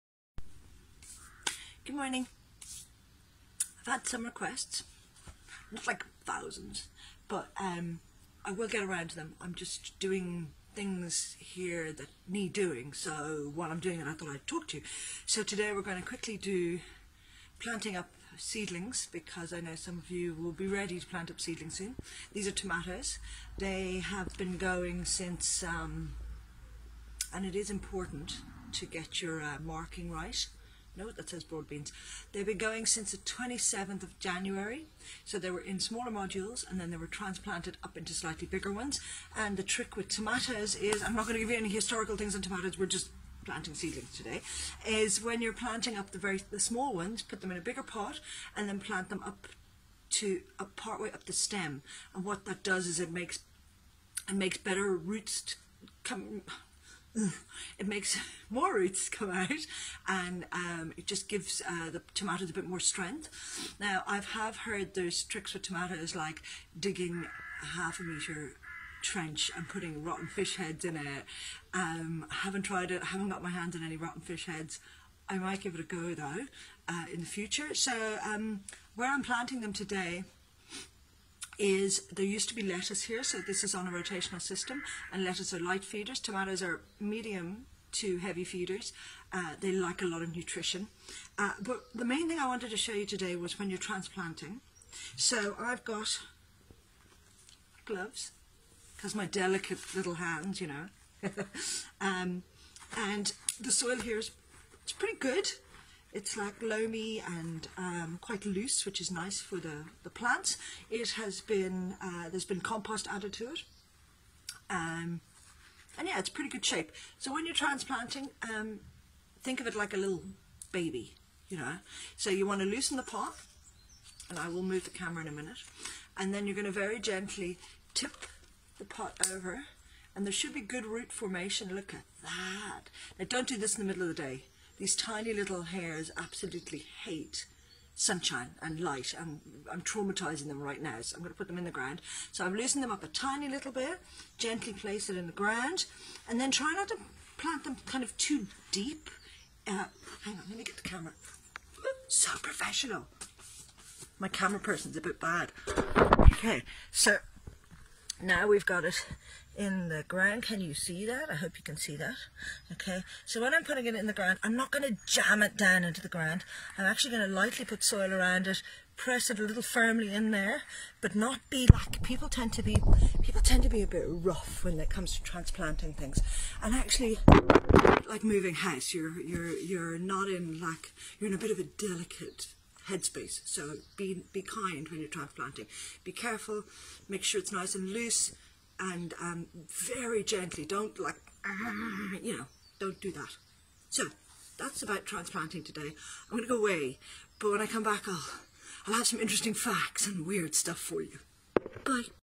Transplanting your Seedlings
A very fast one today as I bored the ass off myself making a cabbage video the other day so here is a quickie on transplanting seedlings. It's neither historical or entertaining however it's useful (and fast did I mention that?!)